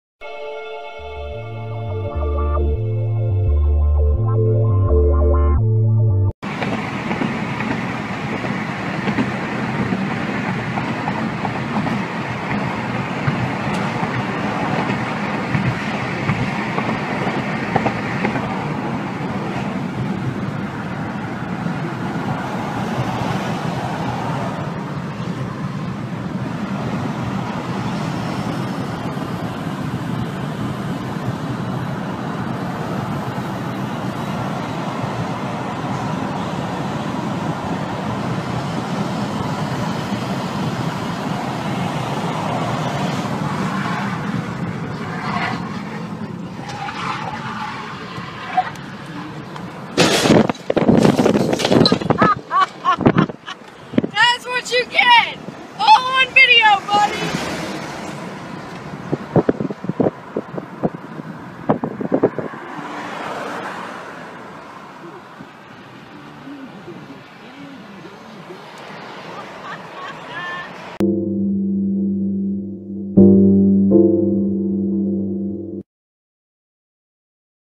Rutube un conducteur le colle et se fait punir
Un conducteur lui colle au cul et se fait punir en le dépassant
Aux États-Unis, le conducteur d’un pickup n’a pas apprécié de se retrouver bloqué derrière un automobiliste et a fini par le dépasser tout en lui faisant comprendre qu’il était furieux. Il s’est vite fait punir…